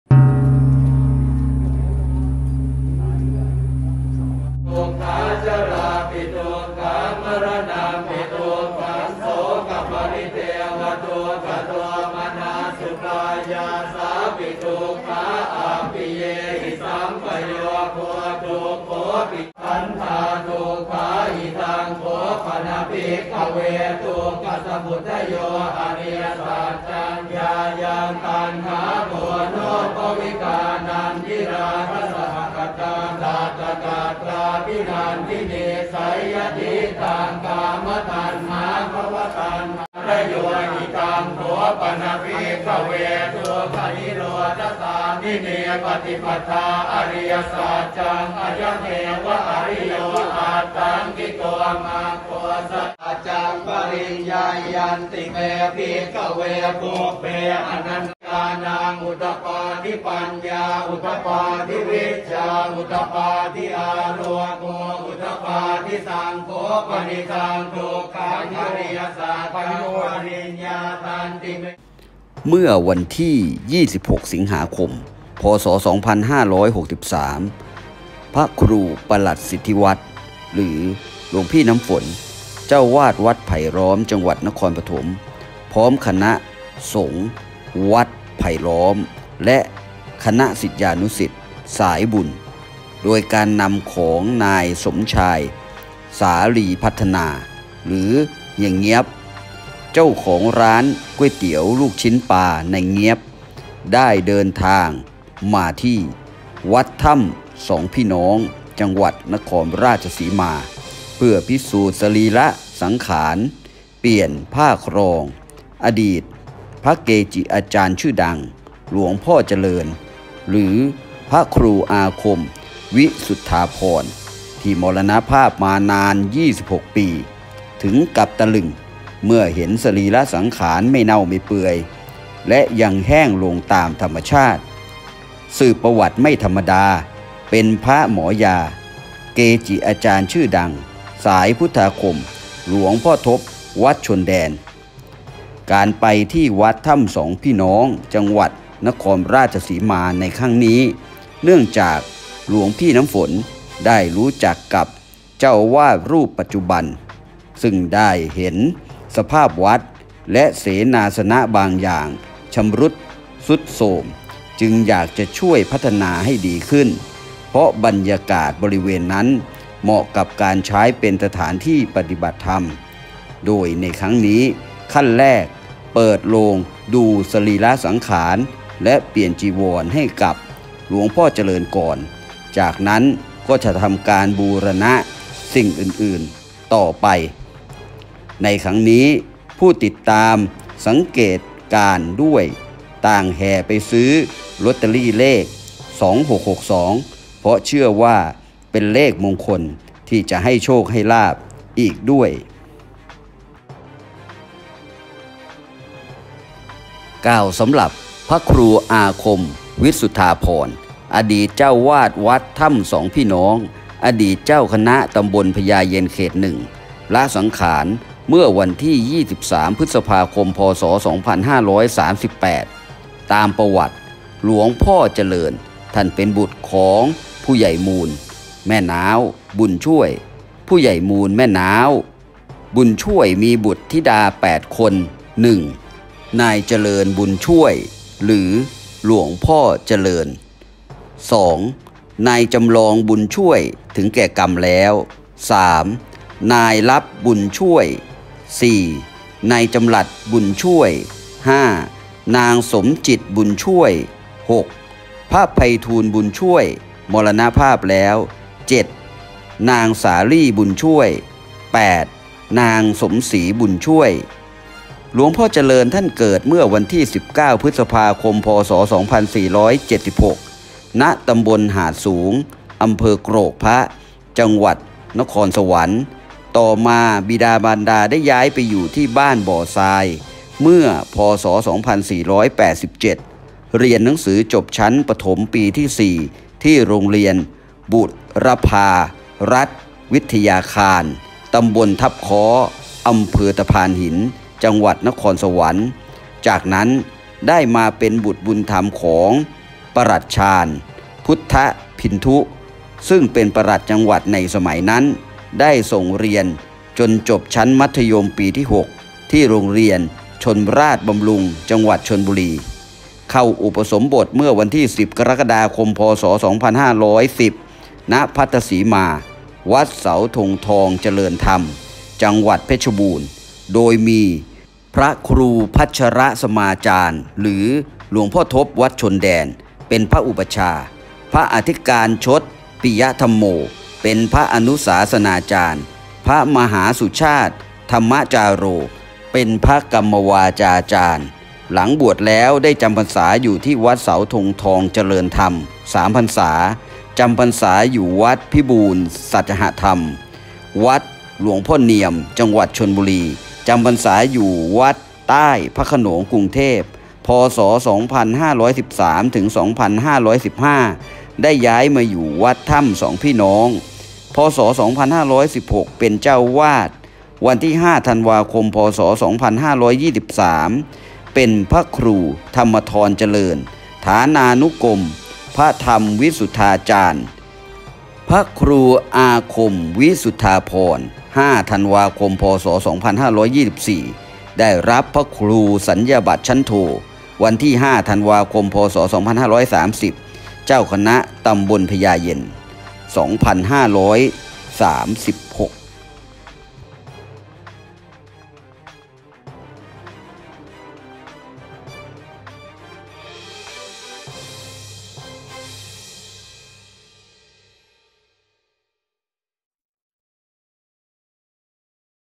บุกพิสูจน์ตะลึง!พระเกจิดังมรณภาพ26ปีสรีระสังขารไม่เน่า
บุกพิสูจน์สังขารเกจิดัง-หลวงพ่อเจริญ ศิษย์สายเกจิดัง”หลวงพ่อทบชนแดน หลวงพี่น้ำฝน-ลุยถึงที่เตรียมบูรณะวัดผู้ติดตามแห่ซื้อลอตเตอรี่เลข 26,62 เมื่อวันที่ 26 สิงหาคม 2563 พระครูปลัดสิทธิวัฒน์ หรือ หลวงพี่น้ำฝน เจ้าอาวาสวัดไผ่ล้อม จ.นครปฐม พร้อมคณะพระสงฆ์ วัดไผ่ล้อม และคณะศิษยานุศิยษ์ สายบุญโดยการนำของนายสมชาติ สาลีพัฒนา (เฮียเงี๊ยบ) เจ้าของร้านก๋วยเตี๋ยวลูกชิ้นปลานายเงี๊ยบ ได้เดินทางมาที่วัดถ้ำสองพี่น้อง จ.นครราชสีมา เพื่อพิสูจน์สรีระสังขารเปลี่ยนผ้าครอง อดีตพระเกจิอาจารย์ดัง “หลวงพ่อเจริญ” หรือ “พระครูอาคมวิสุทธาภรณ์” ที่มรณภาพมานาน 26 ปี ถึงกับตะลึงเมื่อเห็นสรีระสังขารไม่เน่าไม่เปื่อย และยังแห้งลงตามธรรมชาติ สืบประวัติไม่ธรรมดา เป็นพระหมอยา เกจิอาจารย์ชื่อดังสายพุทธาคมหลวงพ่อทบวัดชนแดน
-รายการพระดีเกจิดัง โดย อ.อนุชา ทรงศิริ
วัดไผ่ล้อม, นครปฐม,   พระเก่า, พระใหม่,  พระเกจิ, ลึกลับ, ของขลัง, เลขเด็ด, ไอ้ไข่